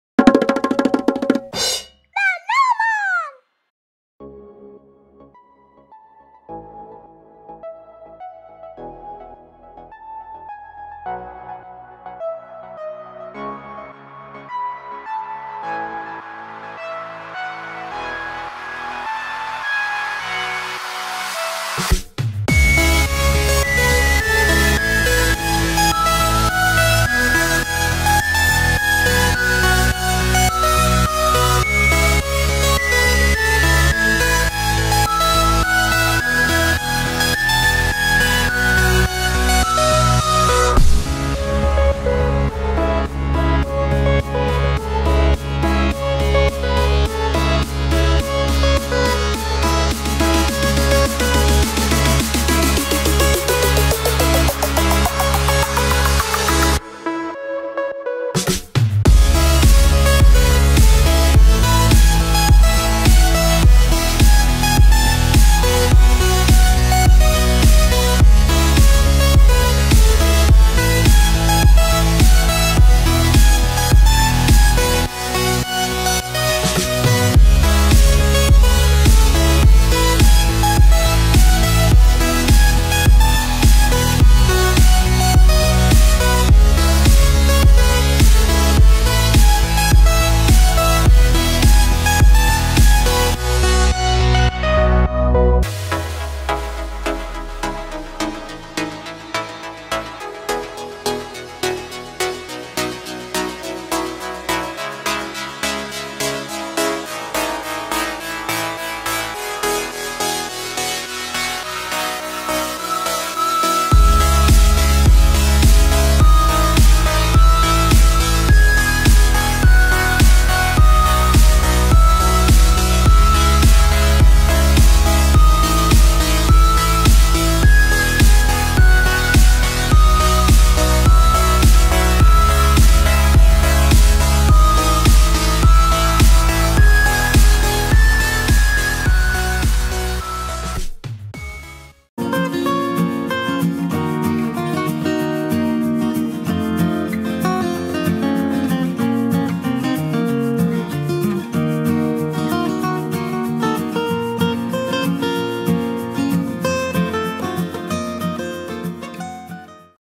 SPEEDPAINT - IBISPAINT X | Wakfu My Hero Academia Fairy Tail Unpolished Fan Art | drawing timelapse
I decided to upload this unpolished digital art I made for my favorite cartoons - Wakfu, My Hero Academia and Fairy Tail. I think they look better with overlapping colors and lines :) .